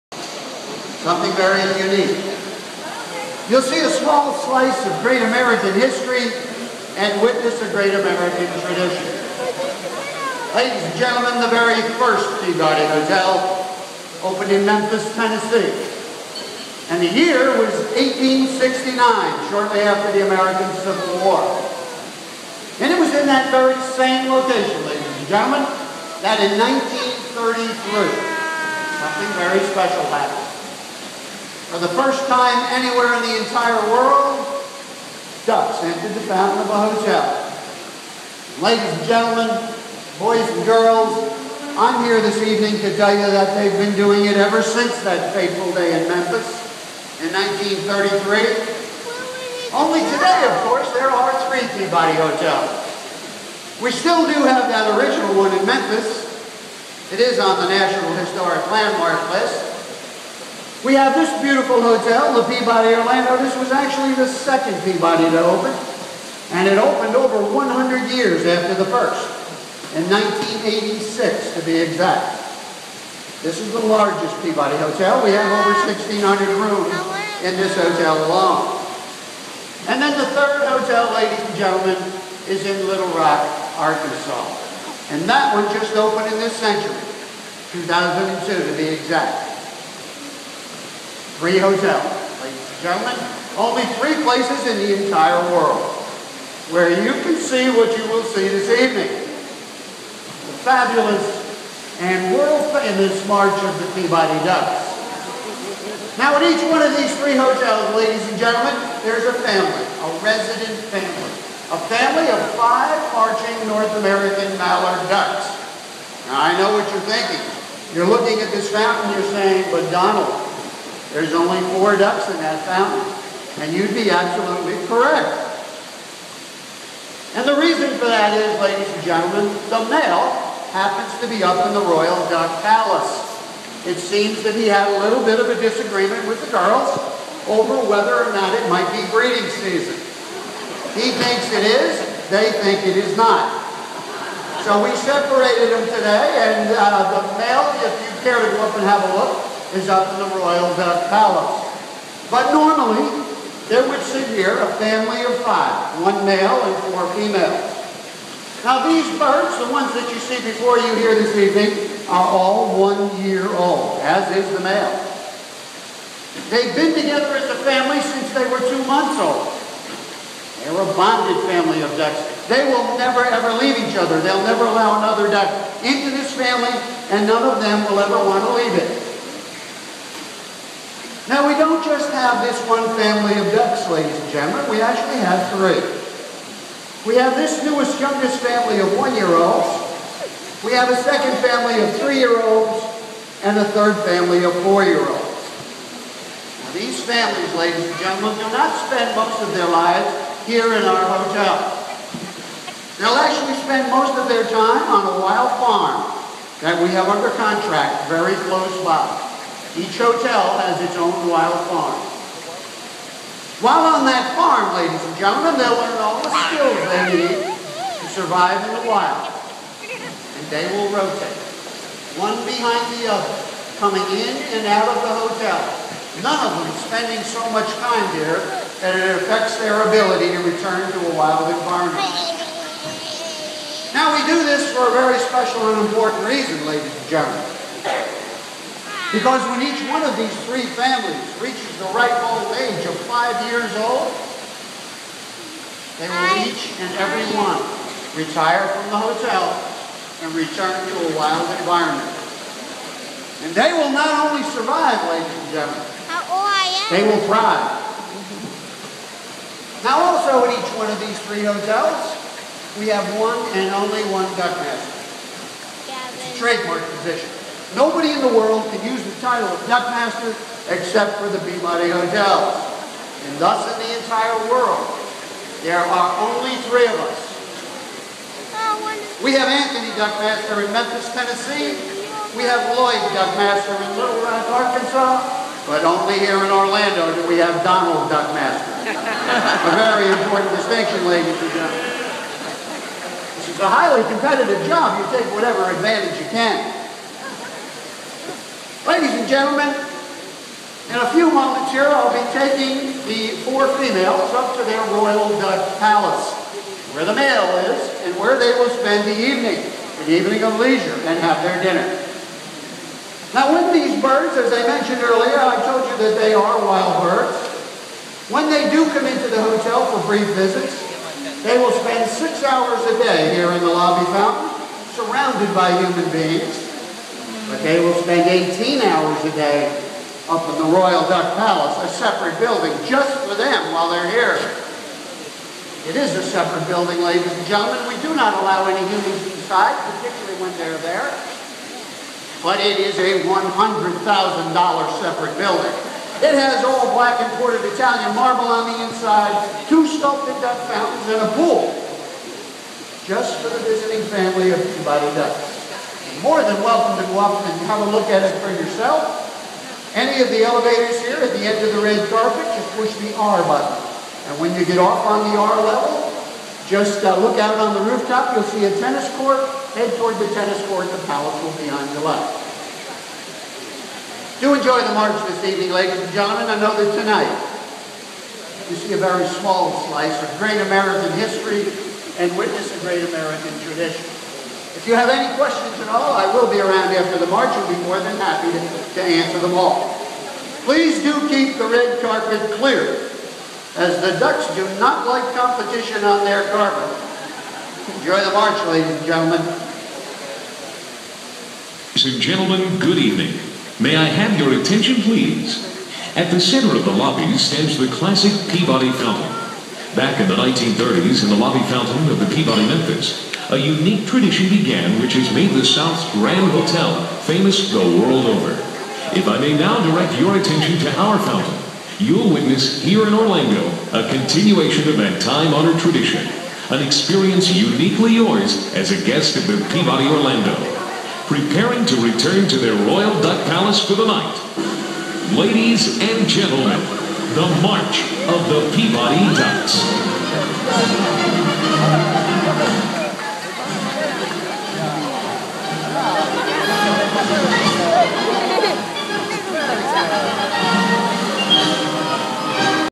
Peabody Ducks introduction and parade
The Peabody Hotel in Orlando, Fl.
Listen to his explanation of their history and watch them march off.
Video by my wife Tracy.